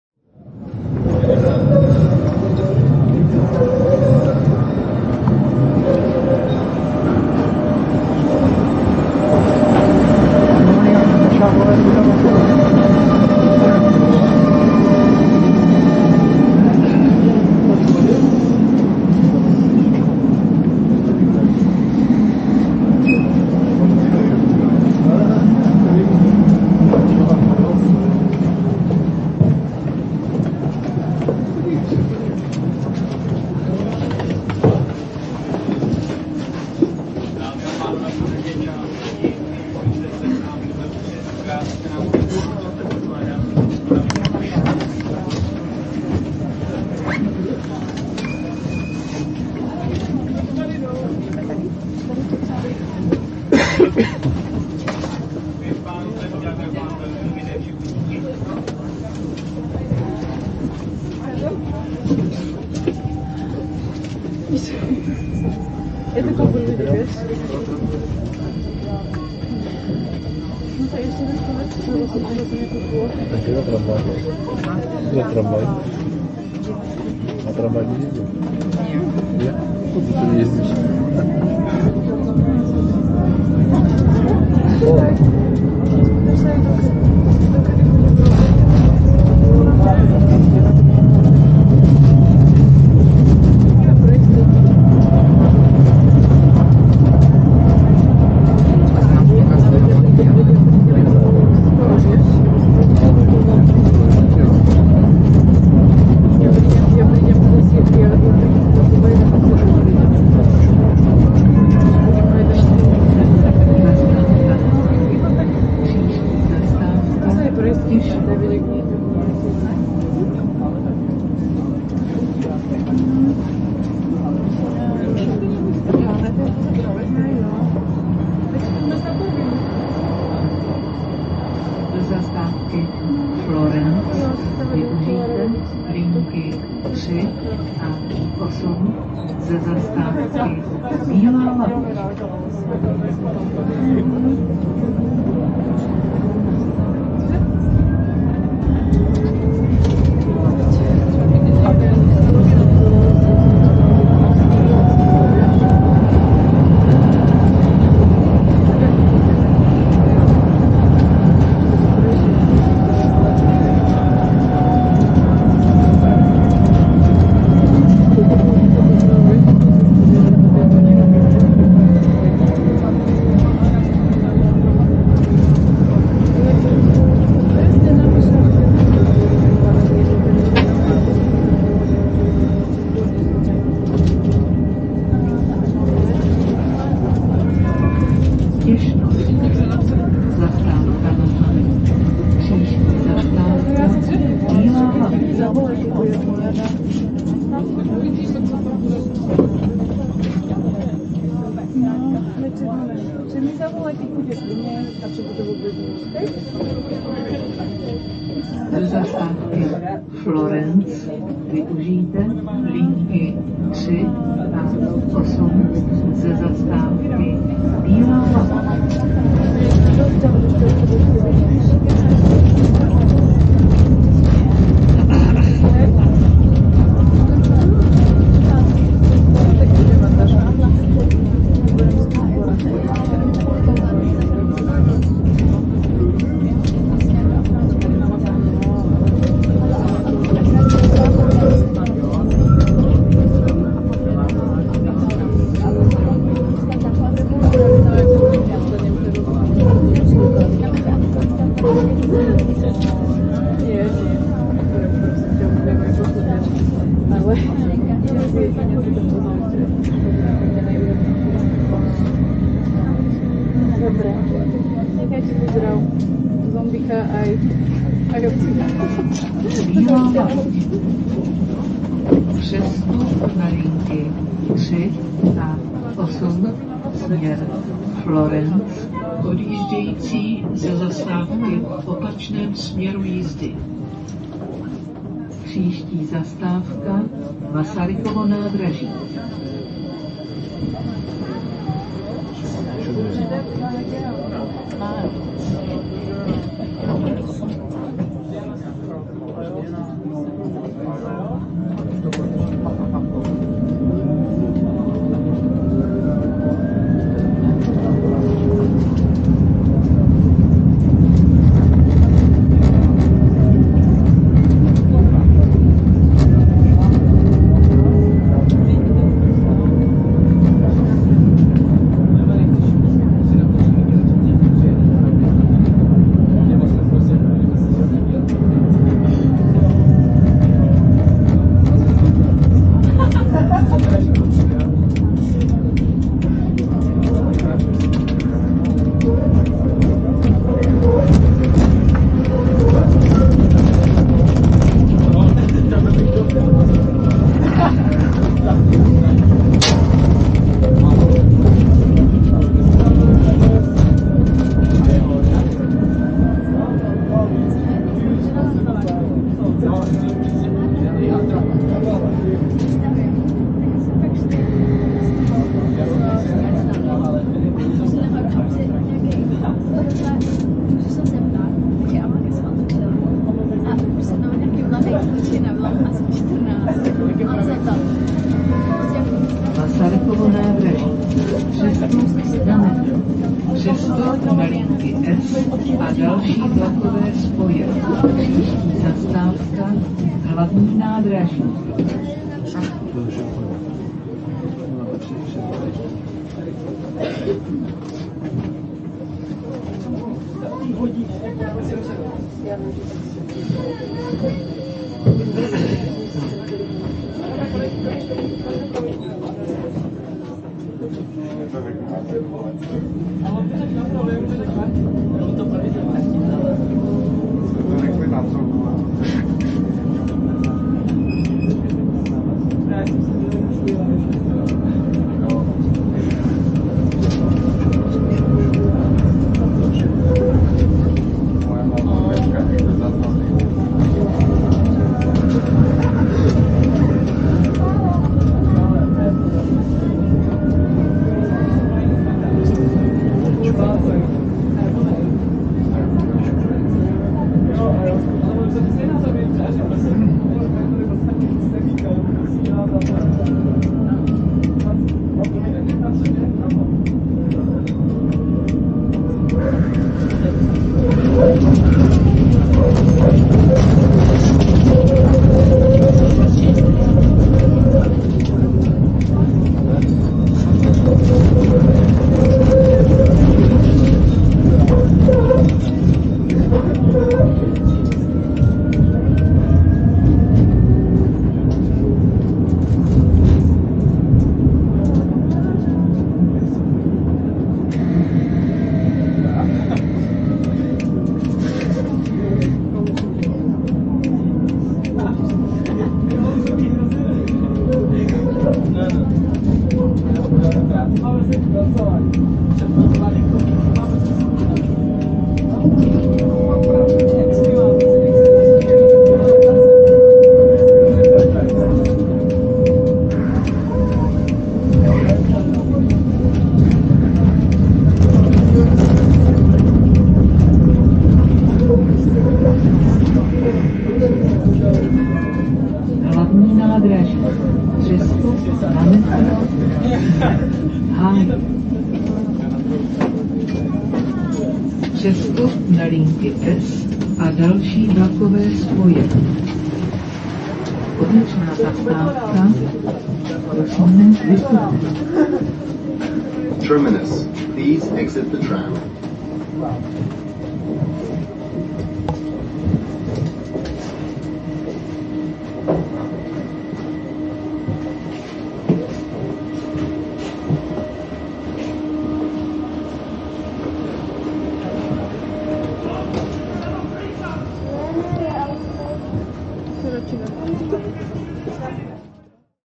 Trams of Prague. Czech Republic. (Part 1).
Video in 360 degree format and 5.7k resolution.
This means that you can twist / rotate it in all directions)

For all those who are not indifferent